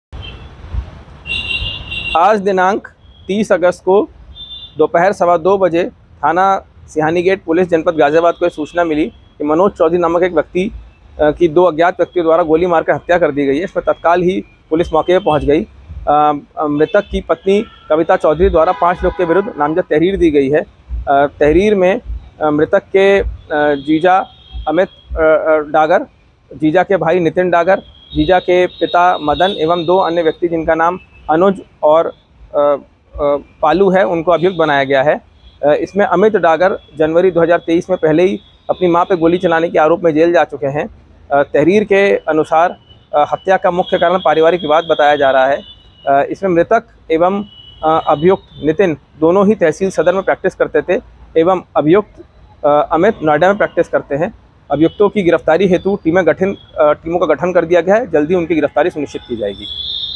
Ghaziabad district Tehsil आज दिनांक 30.08.2023 को थाना सिहानीगेट क्षेत्रान्तर्गत मनोज चौधरी
आज दिनांक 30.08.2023 को थाना सिहानीगेट क्षेत्रान्तर्गत मनोज चौधरी नाम व्यक्ति की दो अज्ञात व्यक्तियों द्वारा गोली मारकर हत्या करने के सम्बन्ध में सूचना प्राप्त हुई ।मृतक की पत्नी द्वारा 05 नामजद व्यक्तियों के विरुद्ध तहरीर दी गई है । (1/2)